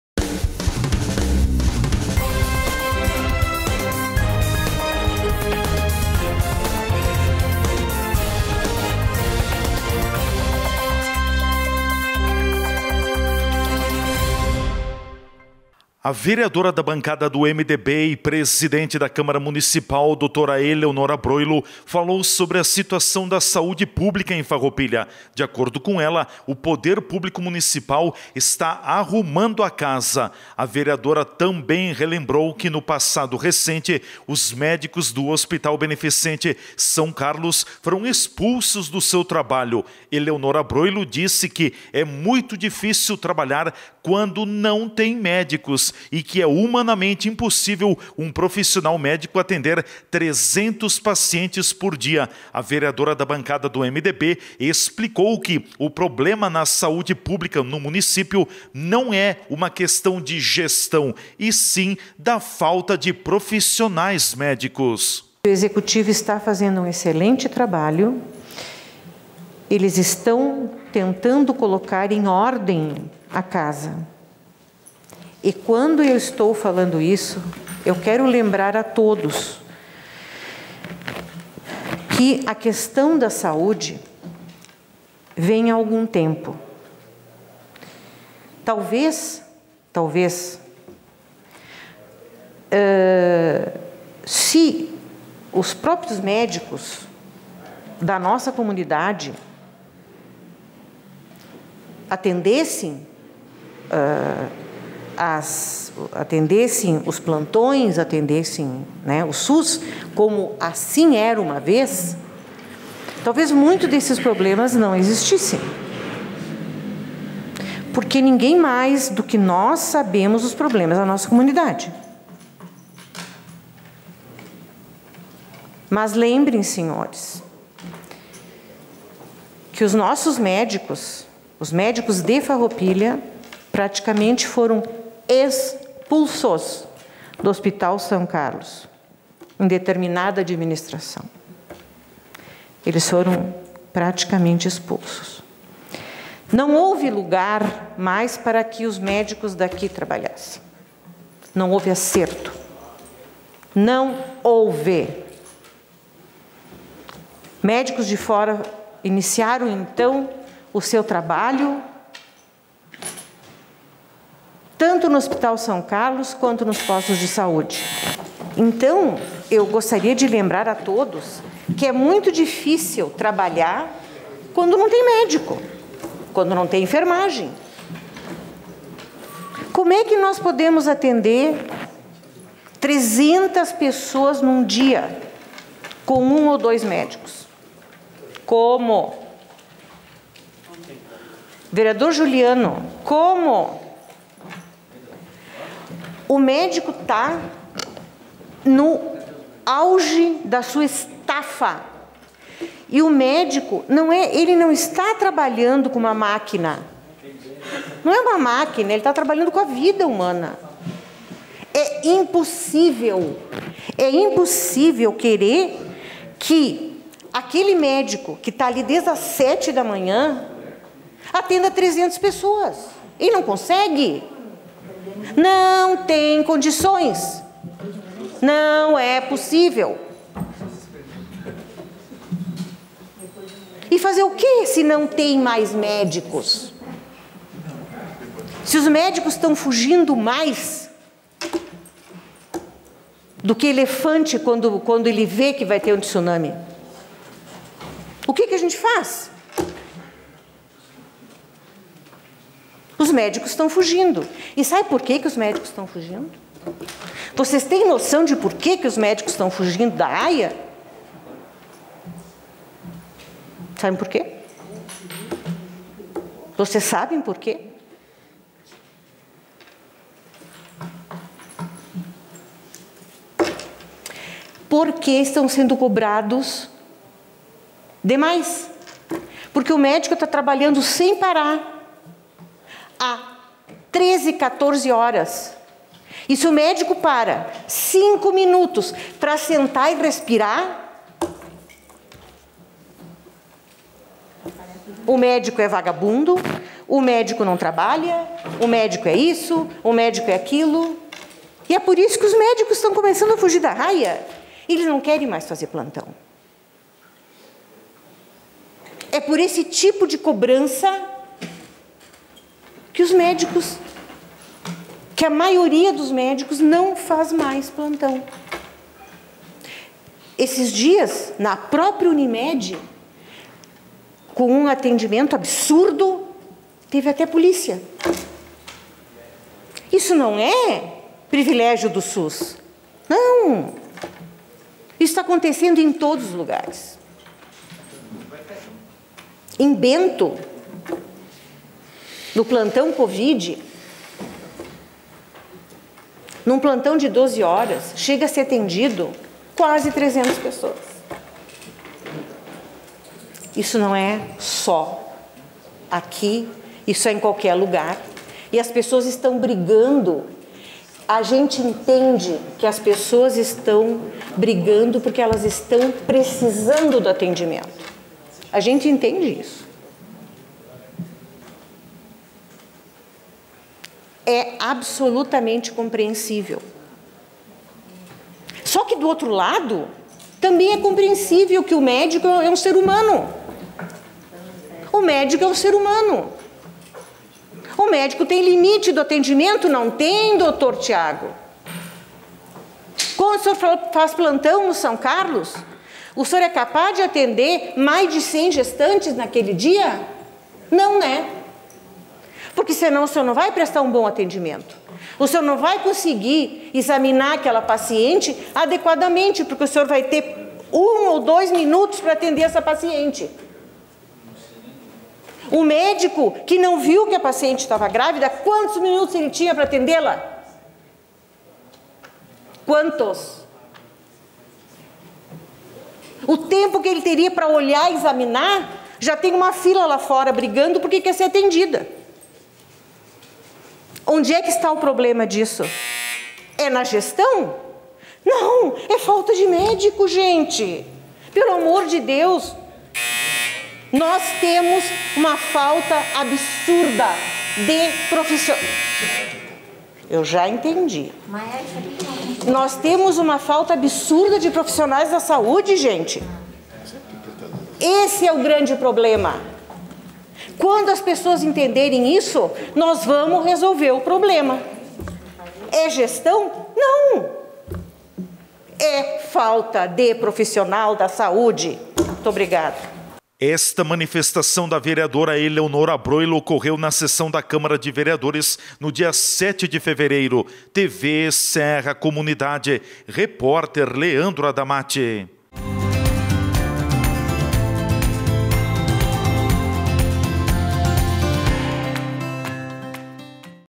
Eleonora Broilo afirma que problemas na saúde pública de Farroupilha não é questão de gestão.
ELEONORA BROILO - VEREADORA DO MDB.
- 14/02/2022.